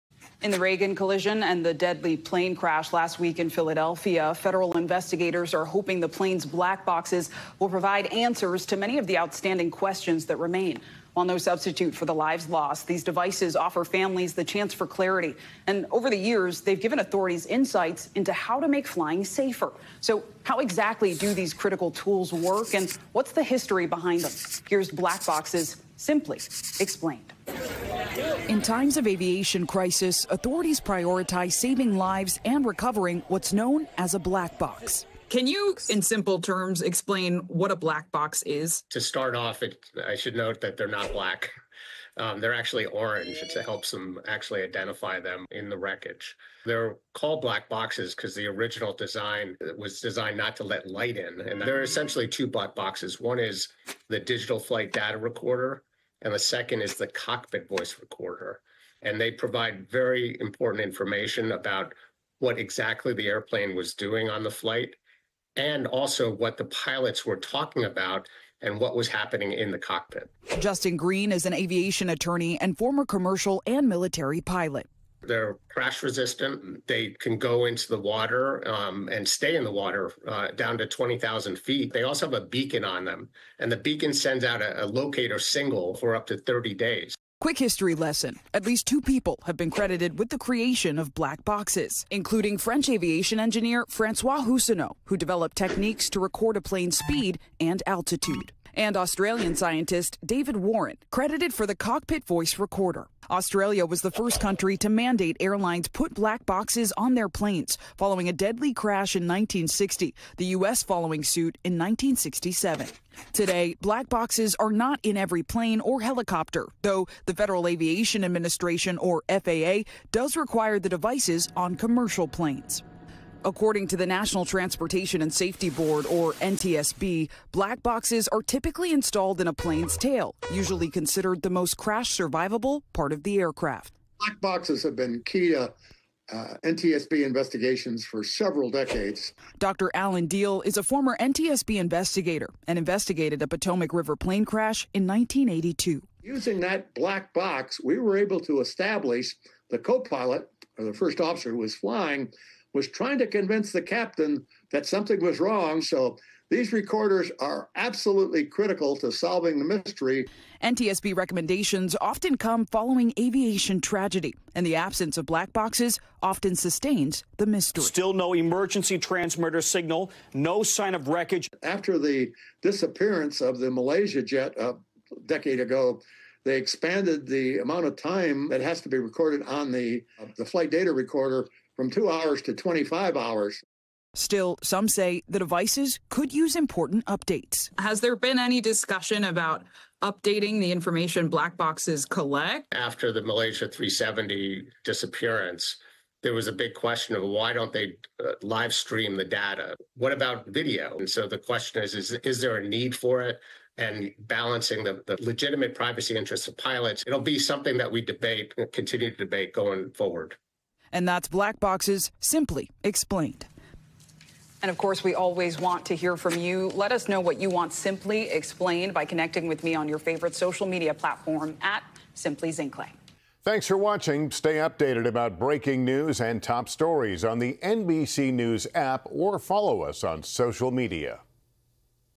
Black boxes explained: The critical role the devices play in aviation
NBC News’ Zinhle Essamuah takes a look at how black boxes work and who invented them in light of the recent mid-air collision over the Potomac River.